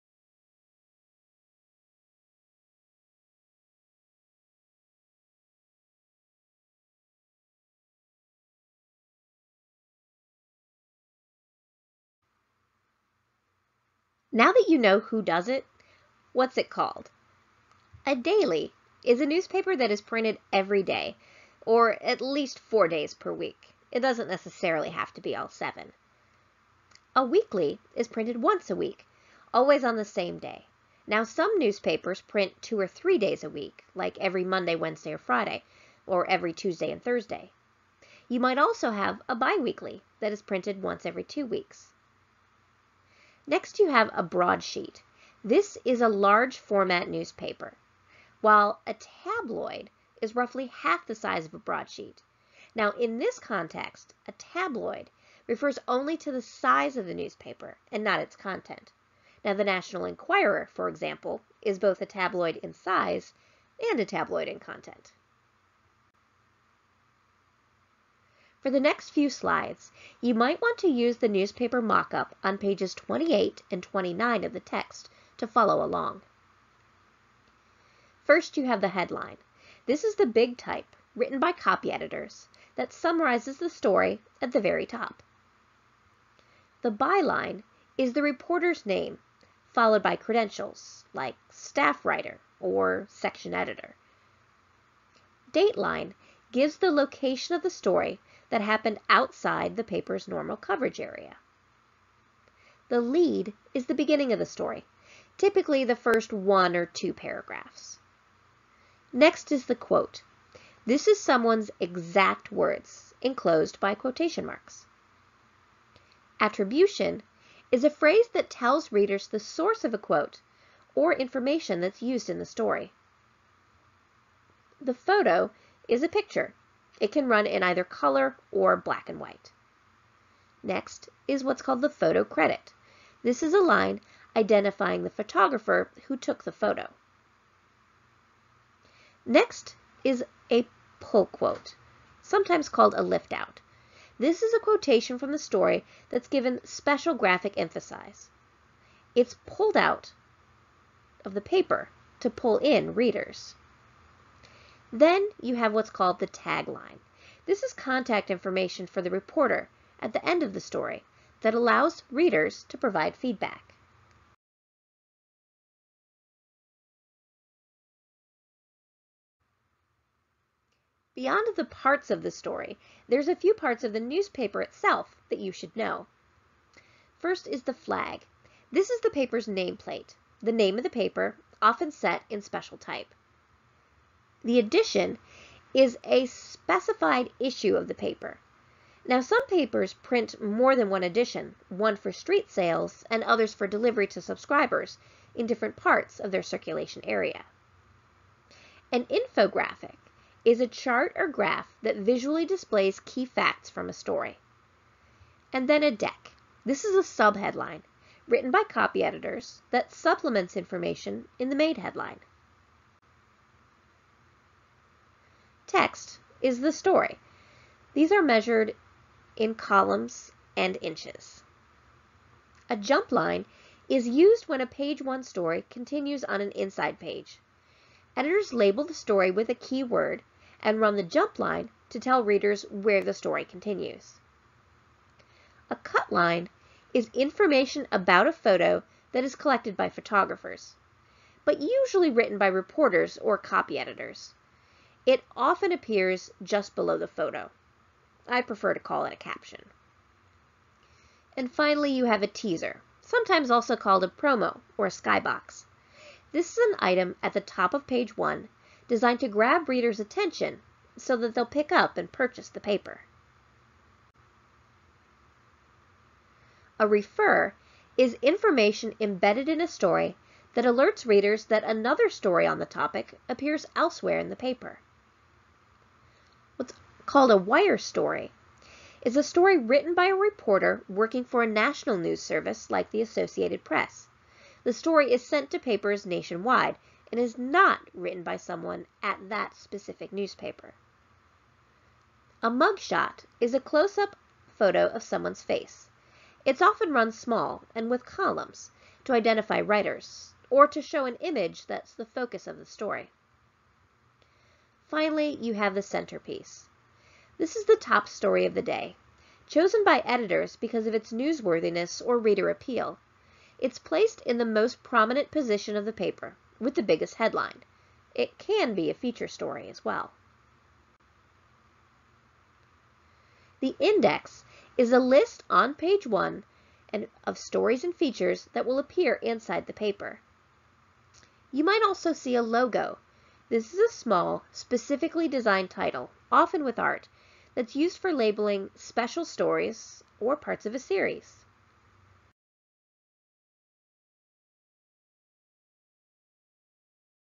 What it's called
Part of a how newsrooms work lecture from Jamie Lynn Gilbert's JOU 216 Mass Media Writing course at Durham Technical Community College.